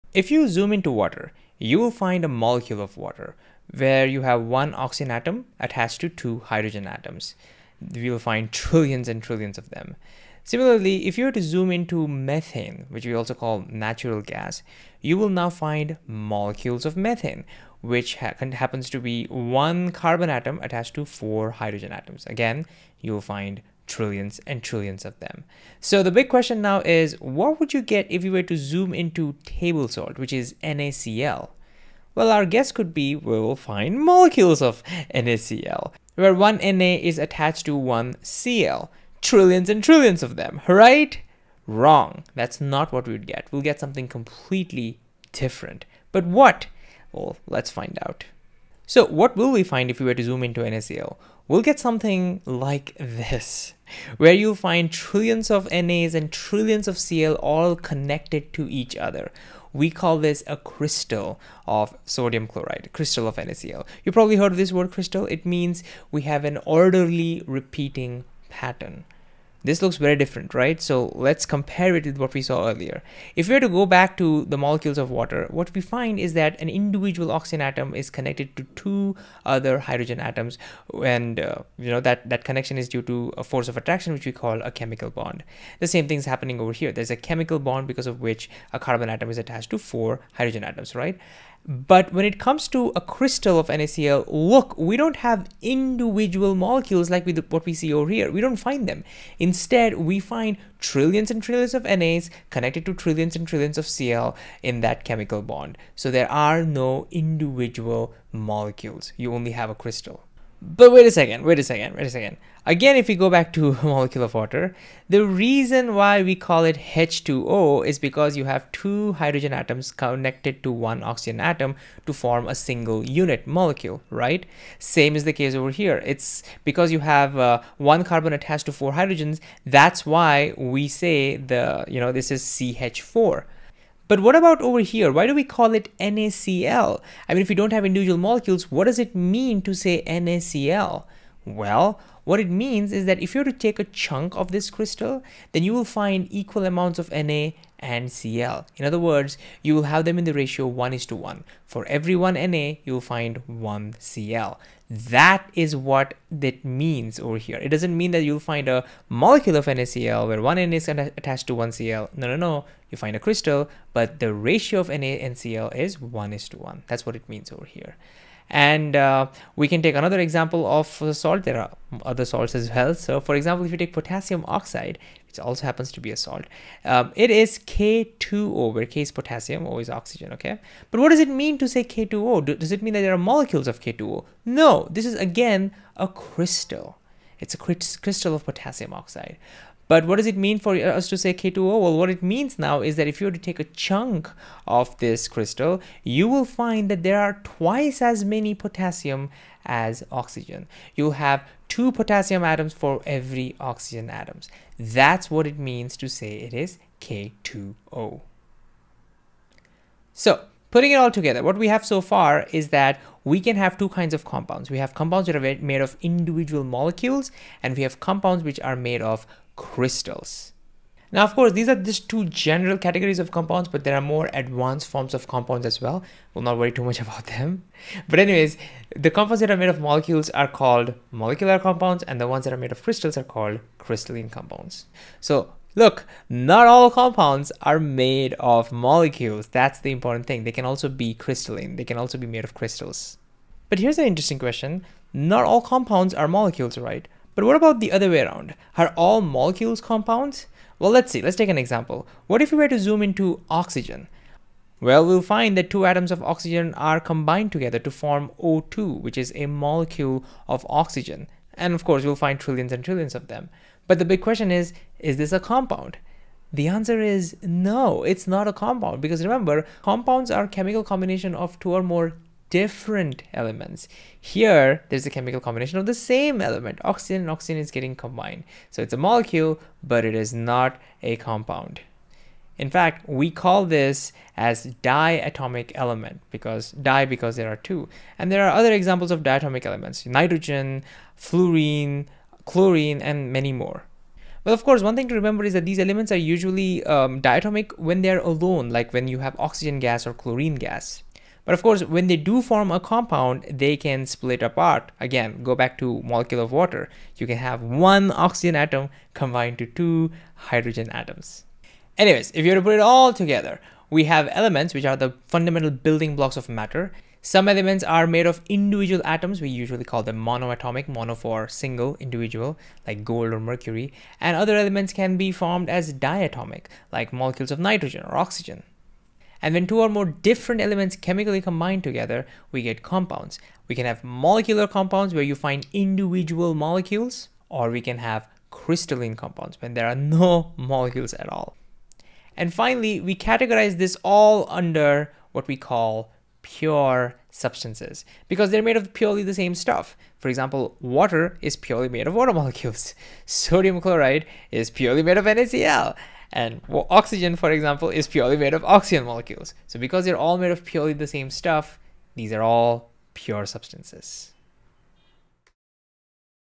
Molecules, crystals, and diatomic elements | Middle school chemistry | Khan Academy
Courses on Khan Academy are always 100% free. Start practicing—and saving your progress—now

Elements and compounds can exist as pure substances, each with a fixed, definite composition. Some compounds, like water, are made of individual molecules. Others, like salt, form crystals—large, repeating 3D patterns without individual molecules. Additionally, some elements pair up as diatomic molecules, such as O₂ and H₂, rather than existing as single atoms. Understanding whether a substance is molecular or crystalline and how diatomic elements differ from compounds is key to classifying matter.

00:00 Intro
00:48 What are crystals?
01:50 Chemical formula of crystals
03:29 General types of compounds
04:03 Diatomic elements
05:11 Summary of pure substances